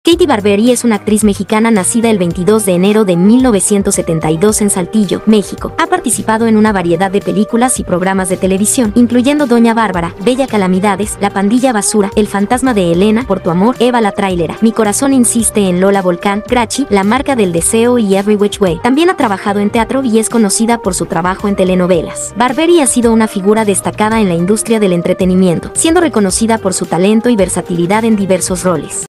😲¡INCREÍBLE TRANSFORMACIÓN DE KATIE BARBERI!
¡Descubre la increíble transformación física de Katie Barberi a lo largo de los años! Desde sus primeras apariciones en 1997 hasta el presente, su evolución es simplemente asombrosa. ¡No te pierdas este viaje en el tiempo lleno de cambios y belleza!